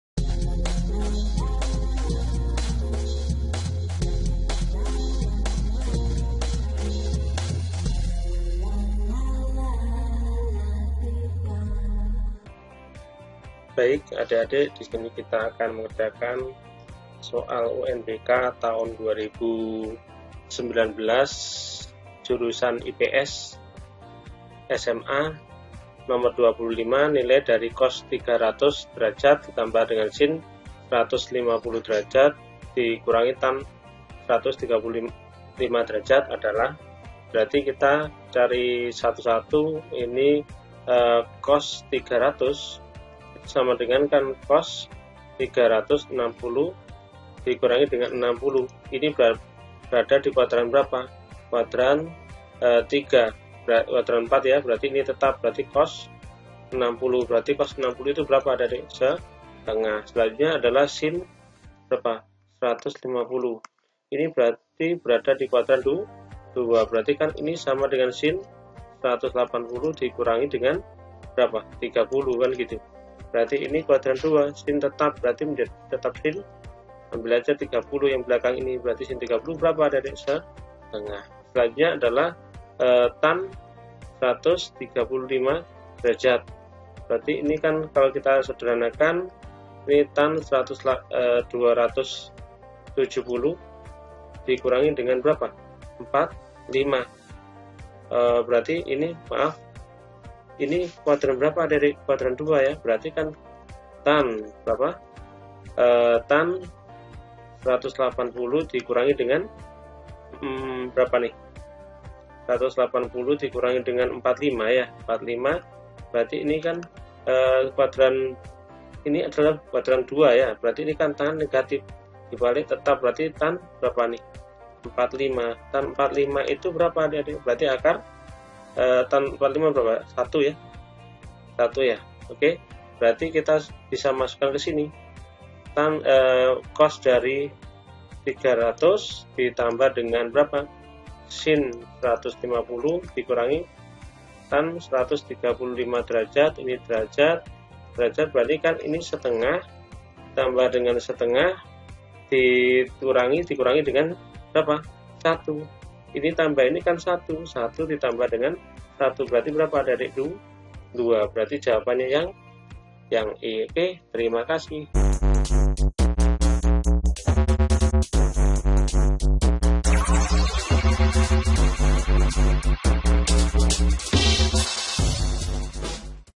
Pembahasan Soal UNBK Matematika SMA IPS Tahun 2019 Nomor 25
Selamat datang para pejuang UNBK 2020, silahkan pelajari pembahasan UNBK Matematika SMA IPS tahun 2019.
Sebelum melihat pembahasan jangan lupa
Pembahasan UNBK Matematika SMA IPS tahun 2019

Silahkan pelajari juga soal-soal di pembahasan Kami selanjutnya
Salam Semangat UNBK 2020